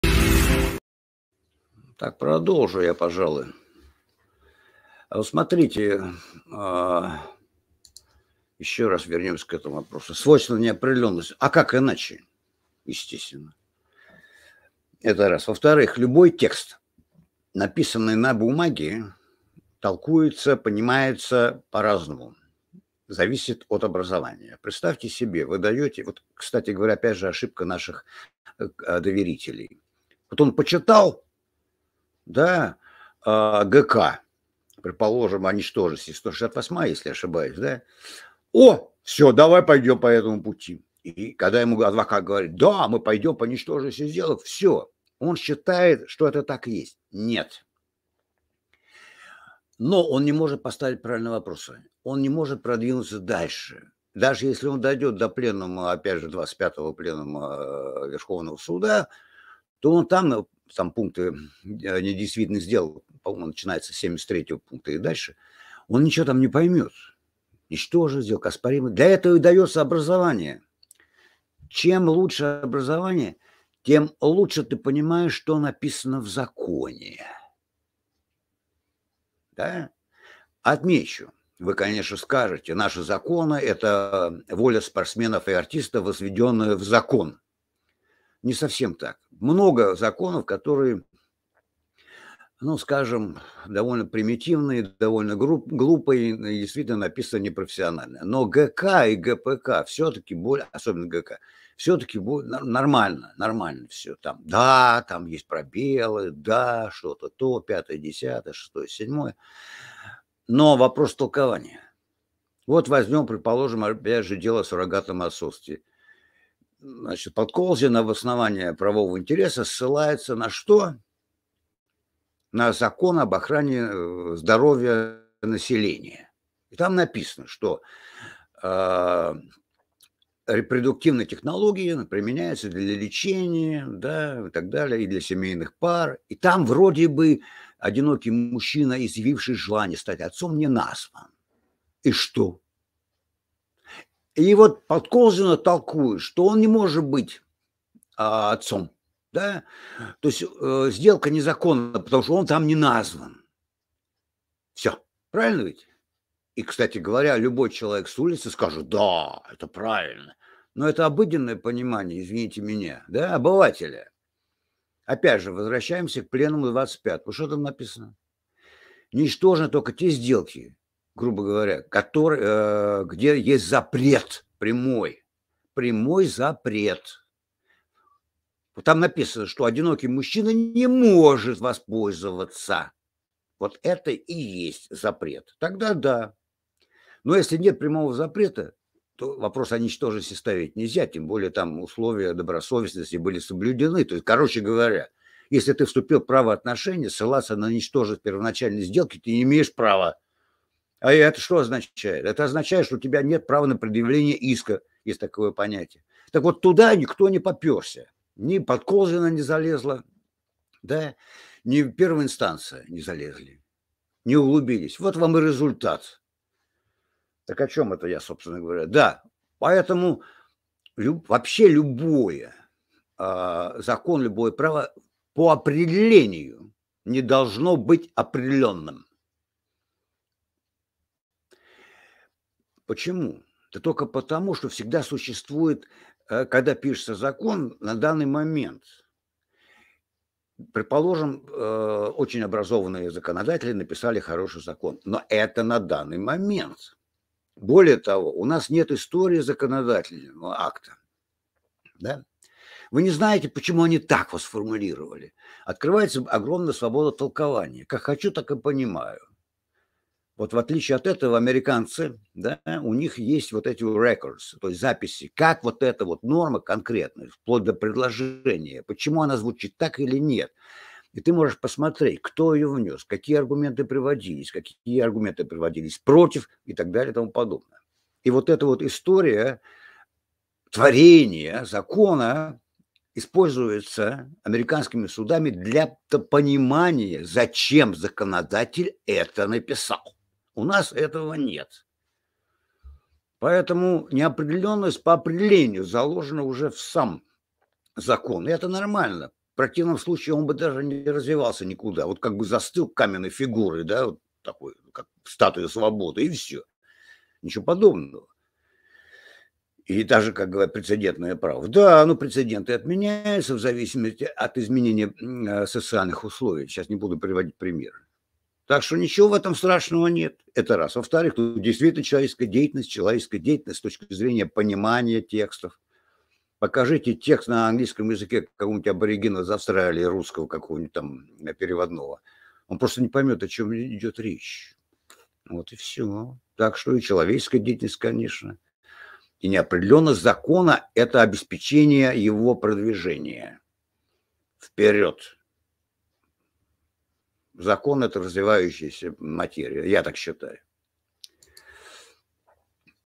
Мера восприятия и понимания человеком
Мера восприятия и понимания.
Написанный текст на бумаге понимается и толкуется
по-разному. Зависит от образования.

Человек, неспециалист в своём деле, не может поставить ПРАВИЛЬНО вопросы, он не может продвинуться дальше.

Чем лучше образование, тем лучше ты понимаешь, что написано в законе.

Наши законы - это воля спортсменов возведённая в закон.

Любой закон, любое право, ПО ОПРЕДЕЛЕНИЮ, не может быть определённым, из-за того, что закон создаётся под конкретный
момент. У нас нет истории законодательного акта. Открывается огромная возможность толкования. Вопросы Зачем законодатель это написал.

Закон - это развивающаяся материя.

 Фрагмент из ролика: "Свободный разговор 26.03.23" от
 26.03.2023 г. Канал: Michael Naked.